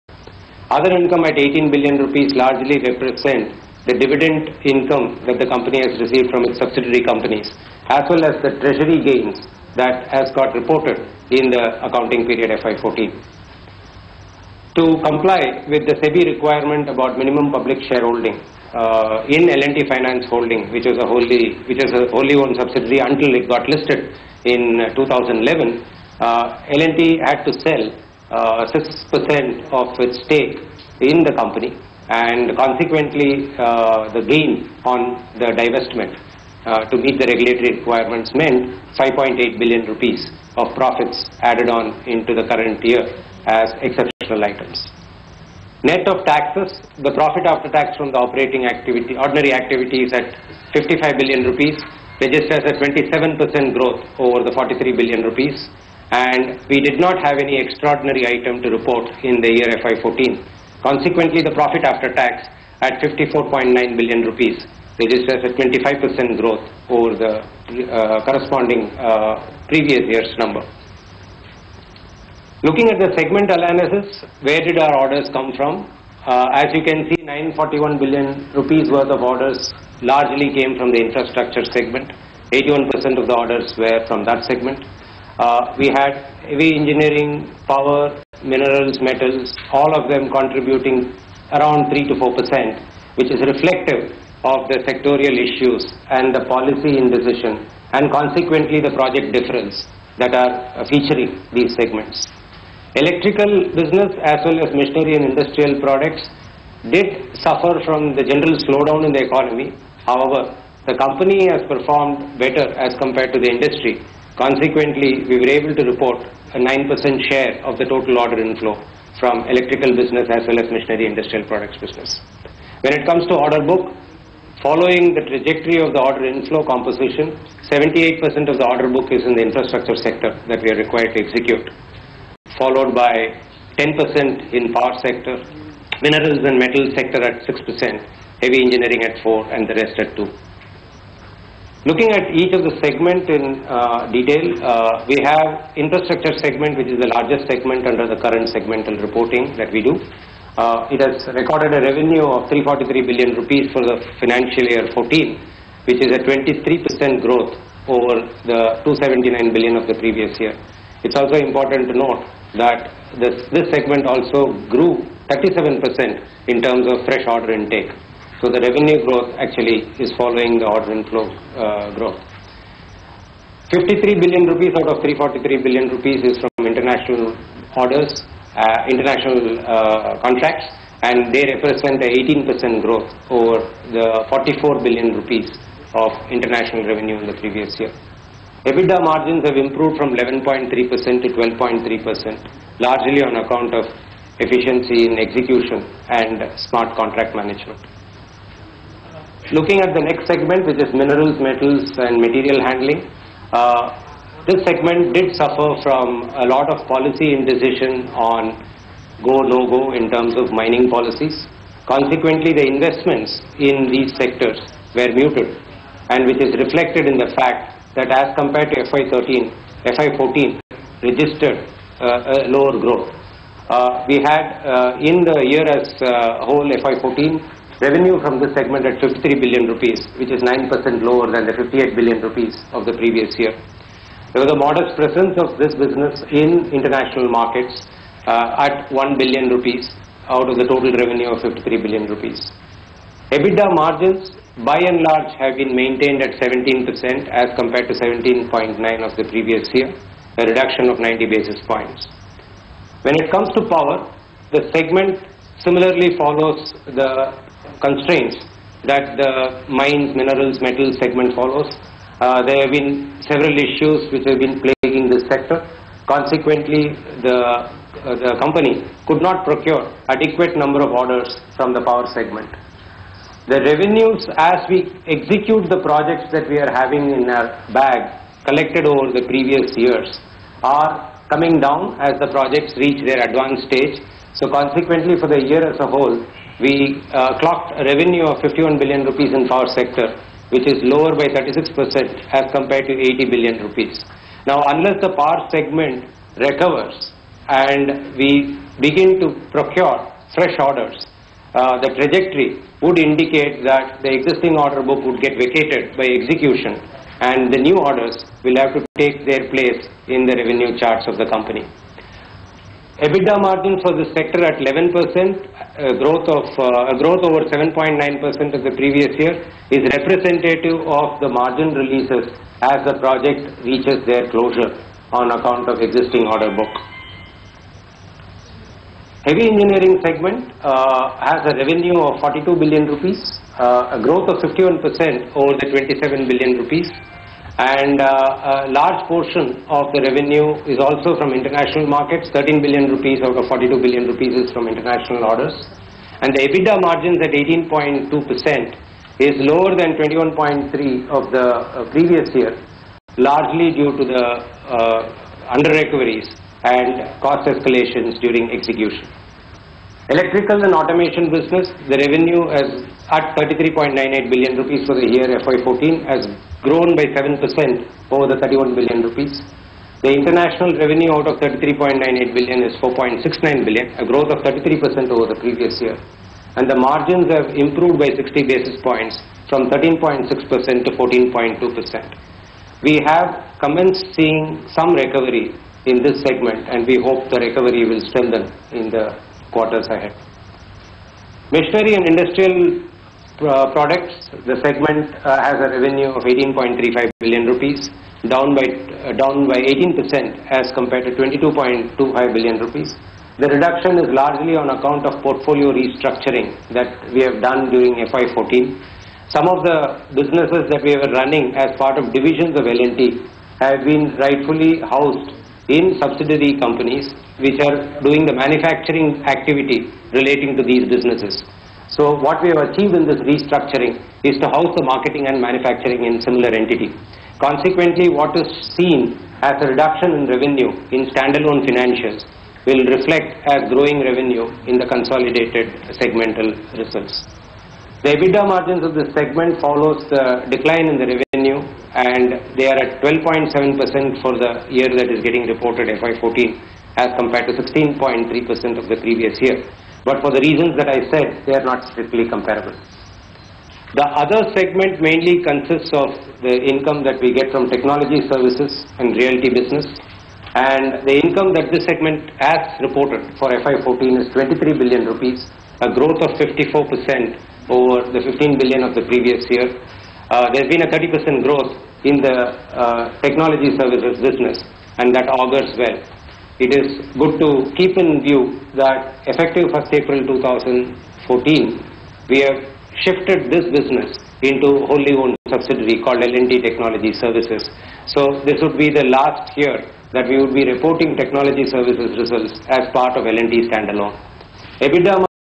L&T Q4 net up 69%, expects FY15 order intake to grow 20% - Part 2
L&T's order inflow for the financial year 2013-14 was up 15 percent to Rs 94,108 crore compared to last year, meeting its guidance.